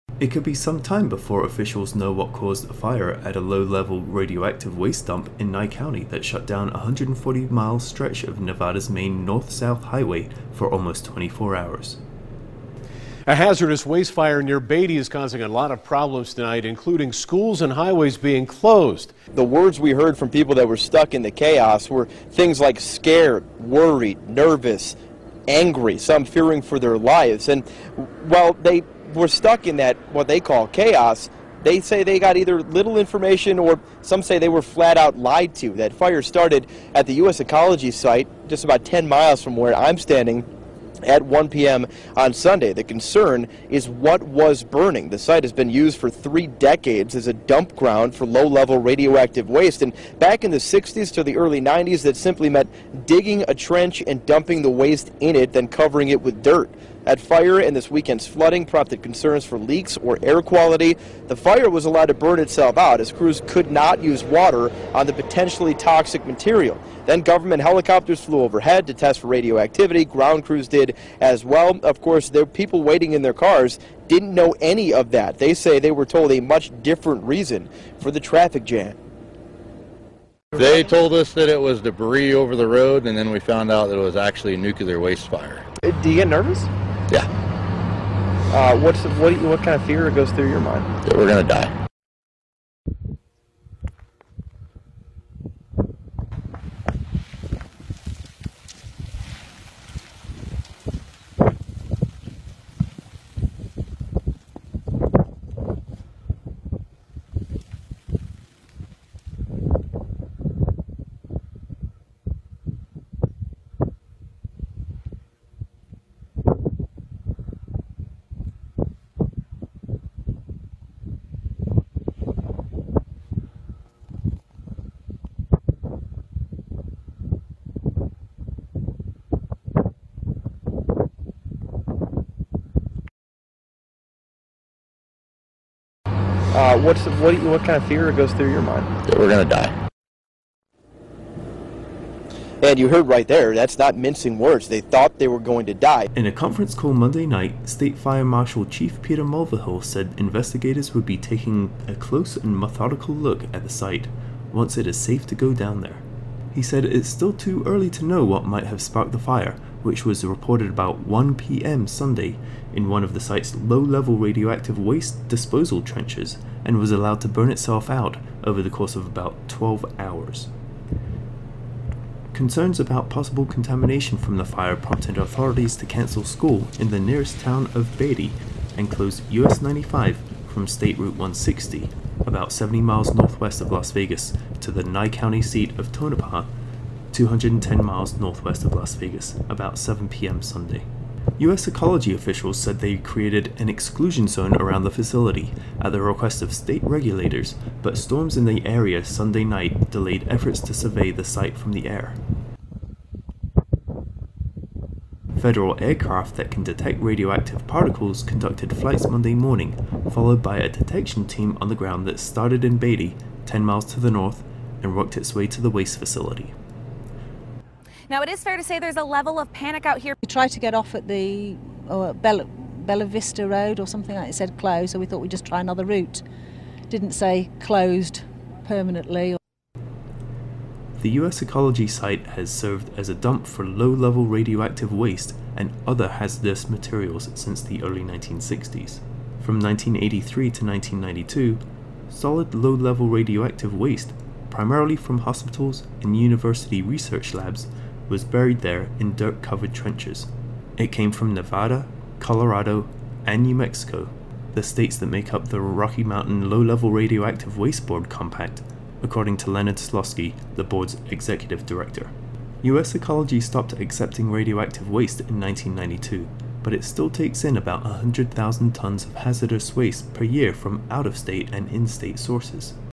☢ Fire at Radioactive Waste Dump by Las Vegas ☠
Nevada Radioactive Waste Facility Fire (October 2015)
It could be some time before officials know what caused a fire at a low-level radioactive waste dump in Nye County that shut down a 140-mile stretch of Nevada's main north-south highway for almost 24 hours.

US Hwy 95 reopened to traffic Monday evening after tests conducted from the air and on the ground showed no signs of radioactive contamination from the blaze that broke out Sunday afternoon at the US Ecology waste site about 110 miles northwest of Las Vegas.

In a conference call Monday night, State Fire Marshal Chief Peter Mulvihill said investigators would be taking a close and methodical look at the site "once it is safe to go down there."

He said it's still too early to know what might have sparked the fire, which was reported about 1 p.m. Sunday in one of the site's low-level radioactive waste disposal trenches and was allowed to burn itself out over the course of about 12 hours.

Concerns about possible contamination from the fire prompted authorities to cancel school in the nearest town of Beatty and close U.S. 95 from state Route 160, about 70 miles northwest of Las Vegas, to the Nye County seat of Tonopah, 210 miles northwest of Las Vegas, about 7 p.m. Sunday.

US Ecology officials said they created "an exclusion zone around the facility" at the request of state regulators, but storms in the area Sunday night delayed efforts to survey the site from the air.

Federal aircraft that can detect radioactive particles conducted flights Monday morning, followed by a detection team on the ground that started in Beatty, 10 miles to the north, and worked its way to the waste facility.

Once it was determined that the fire was out and the surrounding area was safe from radioactive contamination, the highway was reopened about 5:30 p.m. Monday, releasing a flood of vehicles that had been trapped overnight in Beatty.

Mike Harmon, chief of the local volunteer fire department, said all of the motel rooms in town were filled with stranded travelers, and a line of vehicles a half-mile long was waiting at the road block for the highway to reopen.

He guessed there were 30 to 40 tractor-trailers parked along the road and packed into various parking lots in the town of about 1,000 people.

"It's pretty full," Harmon said.

Before the fire, flooding on roads throughout the area had already prompted the Nye County School District to cancel classes Monday in Amargosa Valley, about 20 miles south of US Ecology. The two public schools in Beatty were closed Monday as a precautionary response to the fire, said Cameron McRae, the district's director of transportation, maintenance and operations.

All Nye County schools were expected to open Tuesday.

The US Ecology site has served as a dump for low-level radioactive waste and other hazardous materials since the early 1960s.

From 1983 to 1992, solid low-level radioactive waste primarily from hospitals and university research labs was buried there in dirt-covered trenches. It came from Nevada, Colorado and New Mexico, the states that make up the Rocky Mountain Low-Level Radioactive Waste Board compact, according to Leonard Slosky, the board's executive director.

US Ecology stopped accepting radioactive waste in 1992, but it still takes in about 100,000 tons of hazardous waste per year from out-of-state and in-state sources.

A fire in one of the disposal trenches or in a facility near them would be "very unusual" because there are supposed to be no ignition sources in or around the waste, Slosky said.

Mulvihill said the fire burned a 40-to-50-foot area of the 40-acre site on state-owned land.

As part of their investigation, state officials said they will be looking at the overall stability of the more than 50-year-old dump.

The fire prompted a coordinated emergency response from Nye County and from Nevada's Department of Health and Human Services, Department of Public Safety, Highway Patrol, State Fire Marshal, National Guard, Department of Transportation, and Division of Environment Protection.

The U.S. Environmental Protection Agency also sent an on-scene coordinator to the site.

In 2010, the EPA fined US Ecology nearly $500,000 for 18 hazardous waste violations.

Reports on the violations said that on at least two occasions operators observed smoke coming from a soils decontamination heating unit that resulted in the release of hazardous compounds into the air.

Inspectors also noted "six spills, leaks or other uncontrolled PCB discharges" between 2006 and 2008 that weren't reported as required by law, according to an EPA news release in 2010.

PCBs, or polychlorinated biphenyls, are toxic, organic pollutants that persist in the environment. They were once widely used in electrical transformers and capacitors but are no longer produced in the US

By Henry Brean and Keith Rogers
Las Vegas Review-Journal